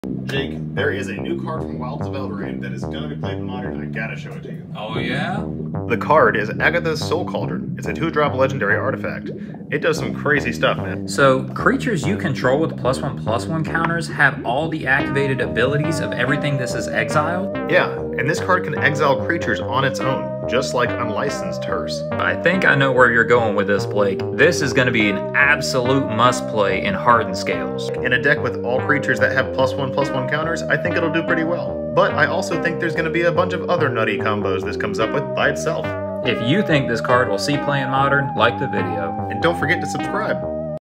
🧹 Crazy Cauldron Calls for COMBOS! 🍯 | Cloudpost Conversations
Jake & Blake highlight a card that already has combo players foaming at the mouth ready to cast it! Agatha's Soul Cauldron has potential to see play in Modern, but let us know how you'd use the card!